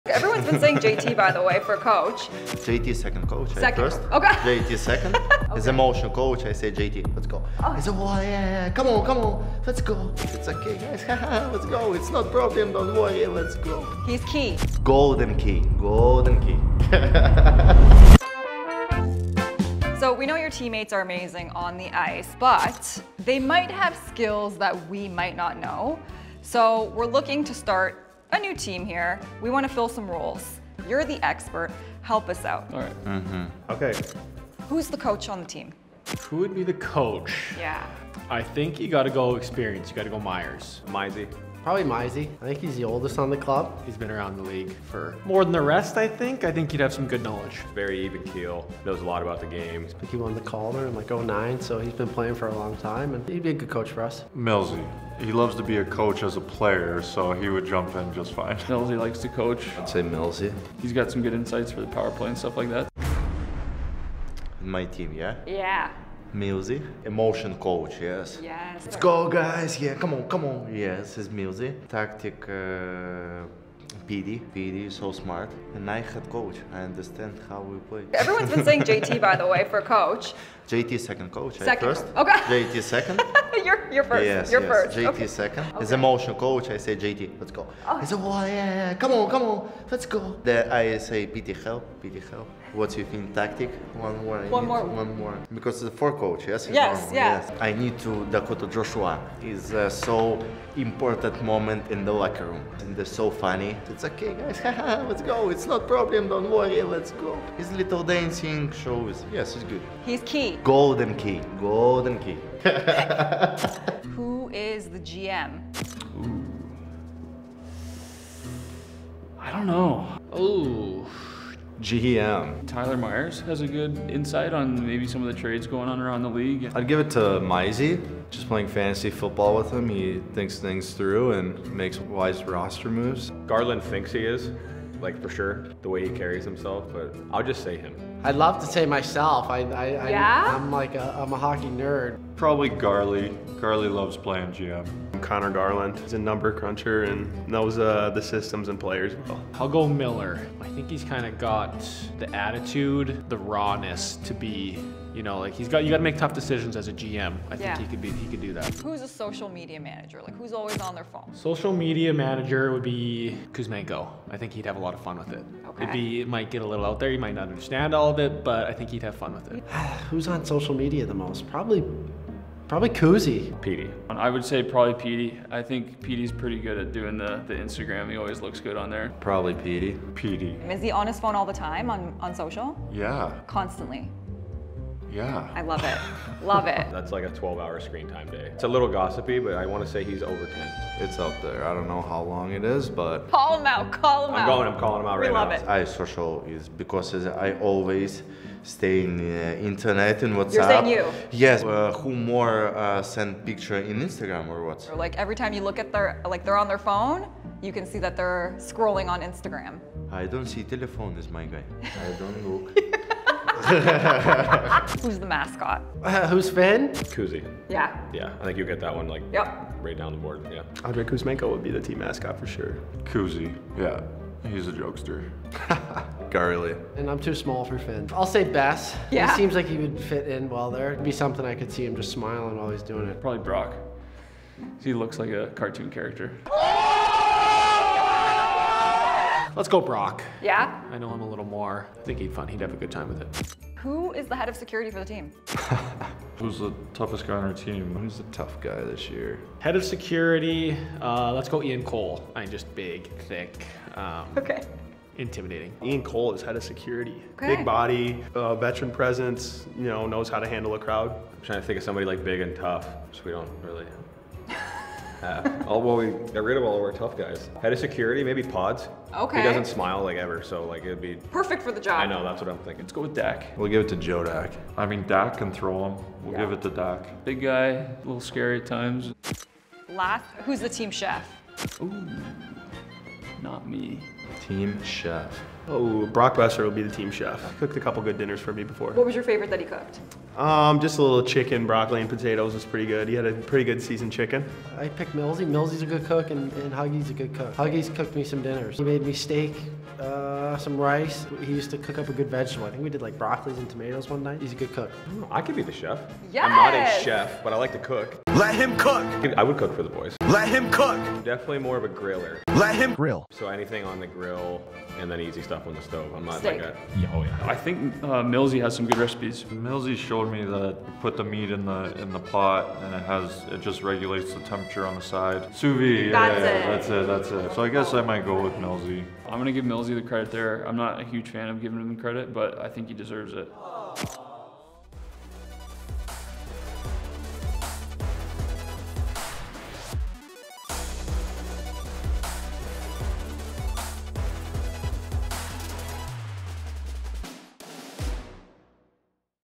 Vancouver Canucks Re-Staff The Franchise With Their Own Teammates
They are at the top of the NHL on the ice, but what about off the ice? Which player on the Canucks would be coach? Who would be the social media manager? Who would be the team chef? Enjoy the Vancouver Canucks chuckling, debating, and even calling out their teammates.

0:00 - Intro
0:22 - Who would be coach?
1:54 - Who would be GM?
2:39 - Who would be social media manager?
3:35 - Who would be mascot?
4:04 - Who would be head of security?
4:40 - Who would be team chef?